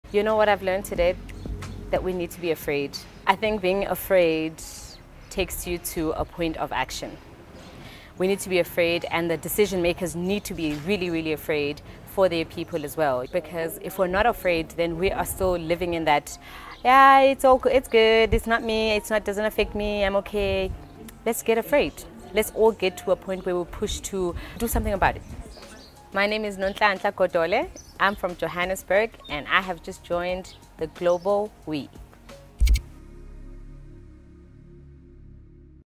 Global We at COP27 - portal in Johannesburg, South Africa
What do you think? Is it fear that will ultimately push decision-makers to finally take climate action? Let’s discuss in the comments.

Thank you Nonhlanhla Godole for visiting our live portal in Johannesburg at @makersvalleypartnership7470 and for joining in meaningful conversations with community leaders and climate activists across the globe.